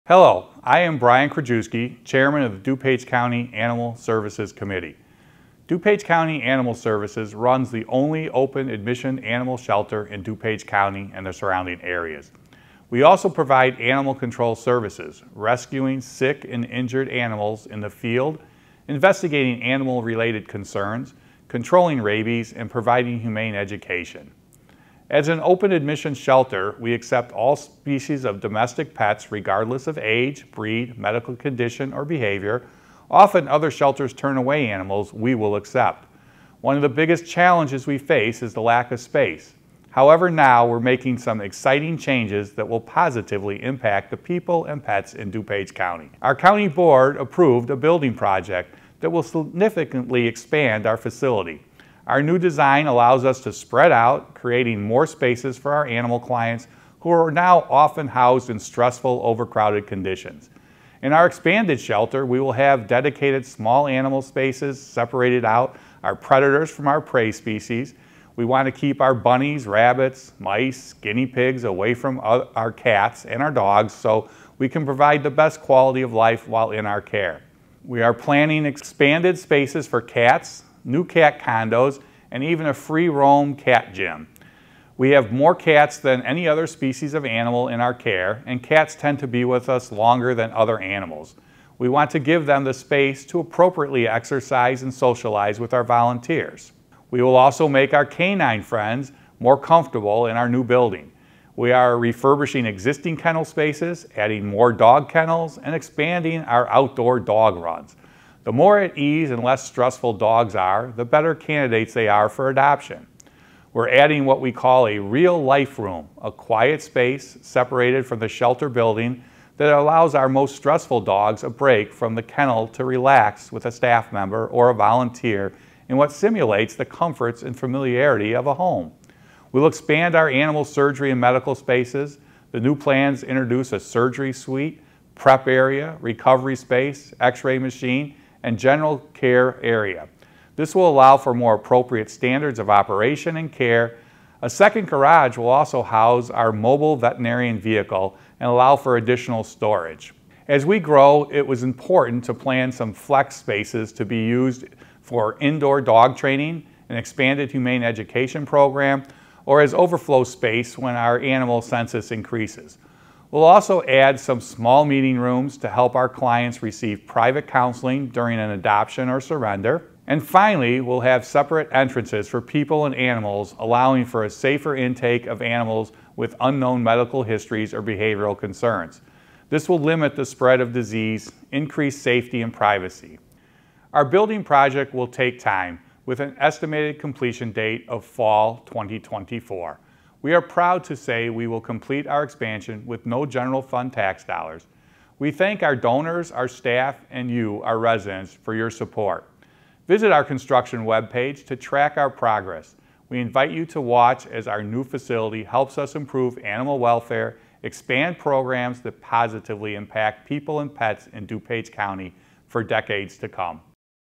DuPage County Animal Services Building Renovation Project
DuPage County Animal Services Chairman, Brian Krajewski, highlights the exciting plans to expand and renovate the DuPage County Animal Services shelter.   July 2023.